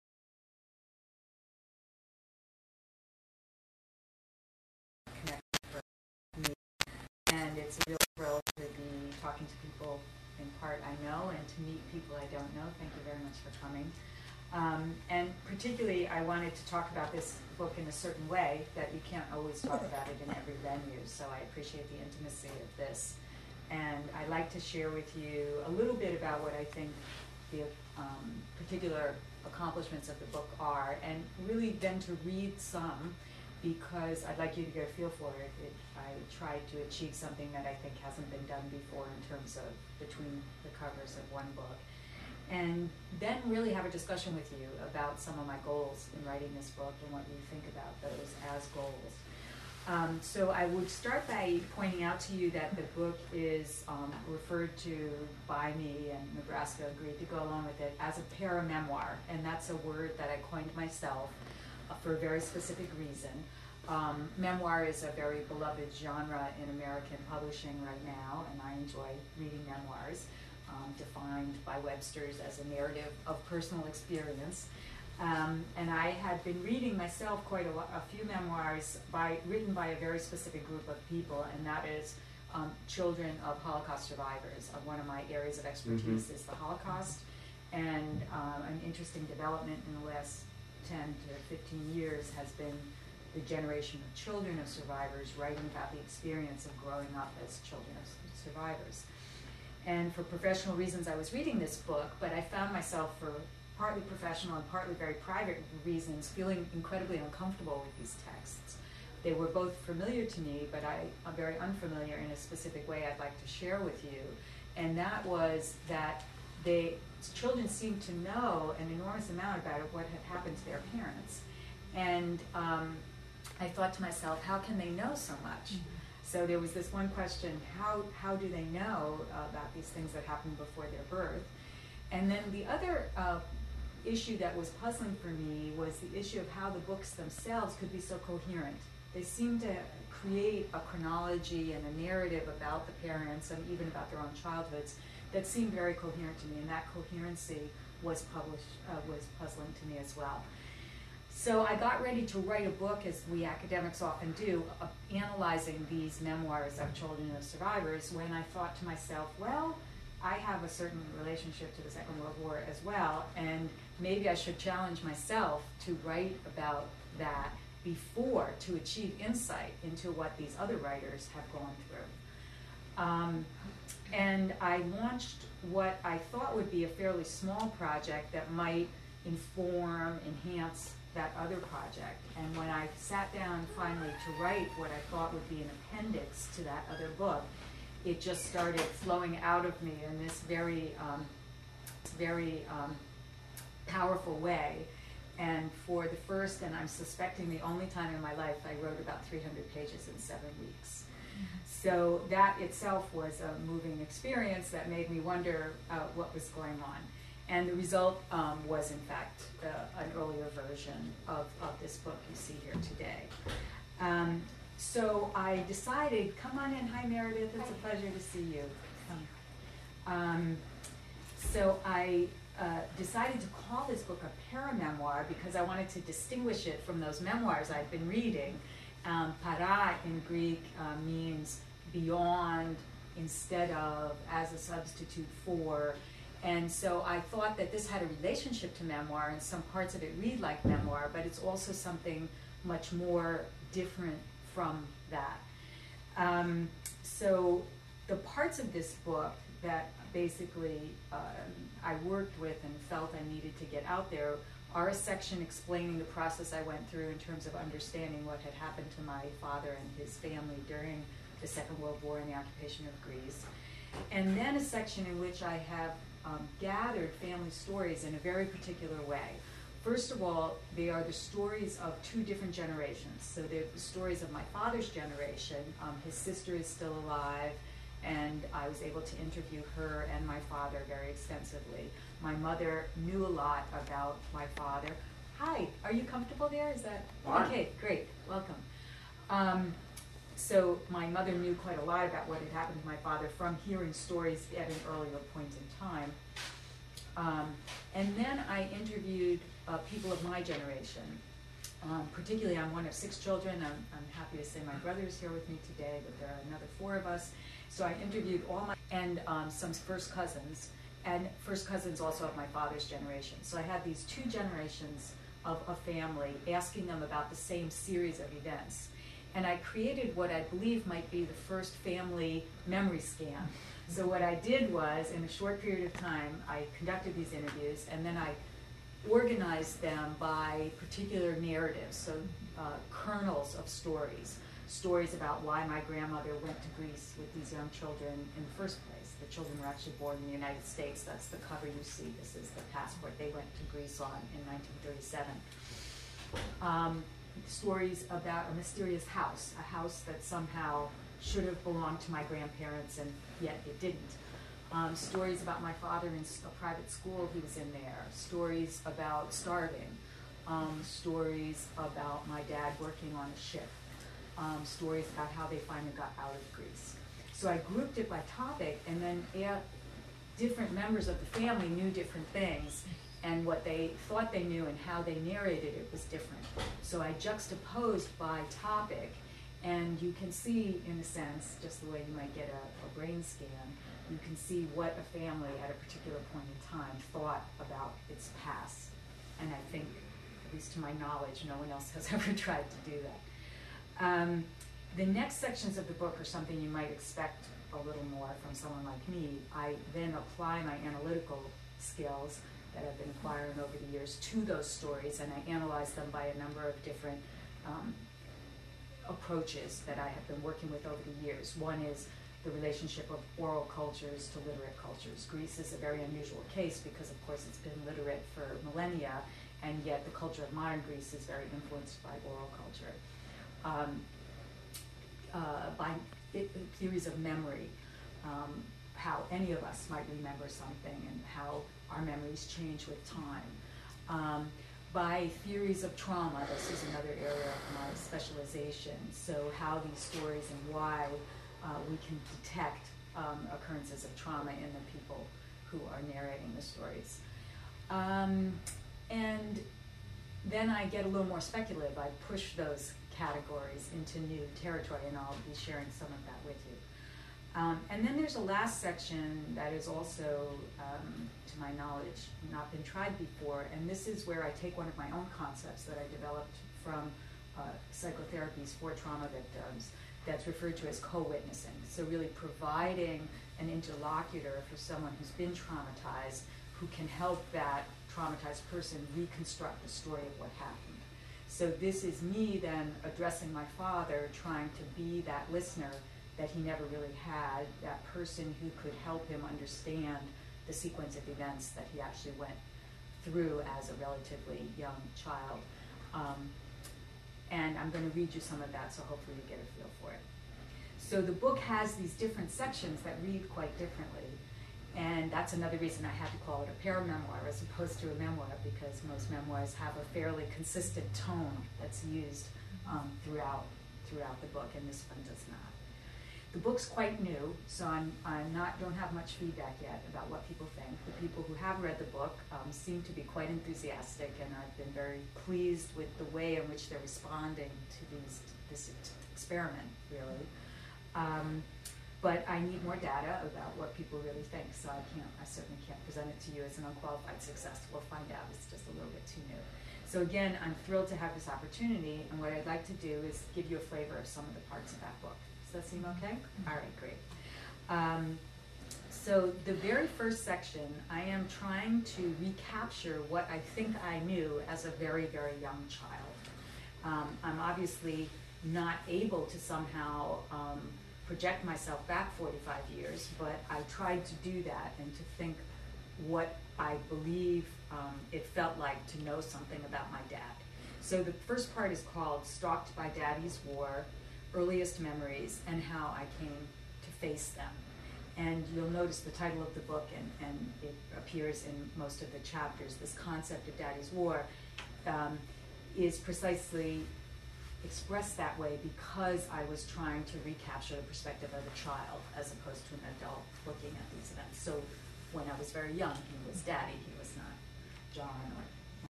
Daddy's War Berkeley Pt1.flv
Part 1 of 6 of the Lecture in Berkeley CA on 5-4-2009 by Prof. Irene Kacandes on her book Daddy's War.